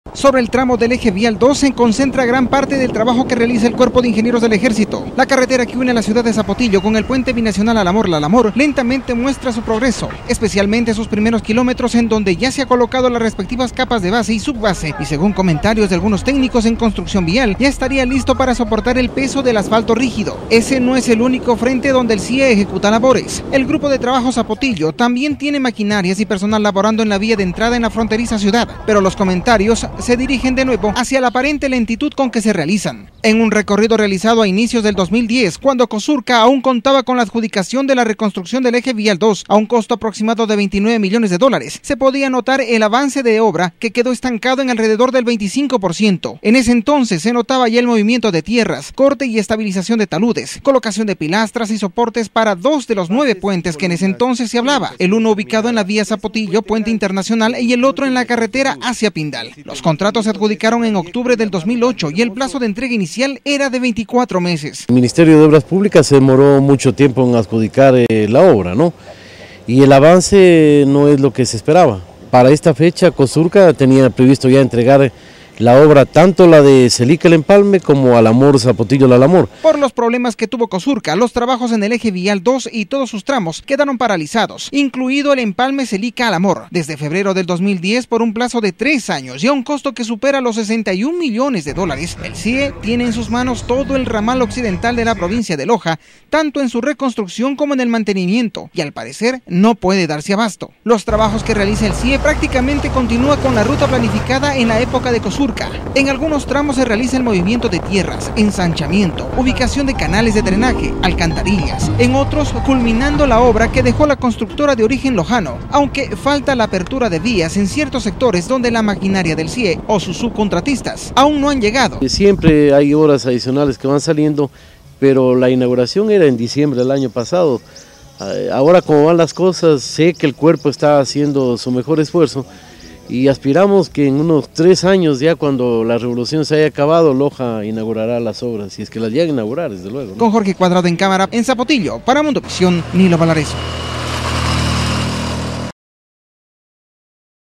VIA ZAPOTILLO LALAMOR
Otro tramo del eje vial 2 que está a cargo del Cuerpo de Ingenieros del Ejército es el que conectará a Zapotillo con el puente internacional en Lalamor. A pesar de que en esta vía se concentran mayoritariamente el flujo de actividad de trabajadores y maquinaria, los trabajos podrían demorar más de lo esperado. Cosurca dejó cerca del 25% de la obra adelantada.
(incluye imágenes de archivo 2009)
REALIZACIÓN: Nilo A. Valarezo M., para Ecotel Televisión
CÁMARA: Jorge Cuadrado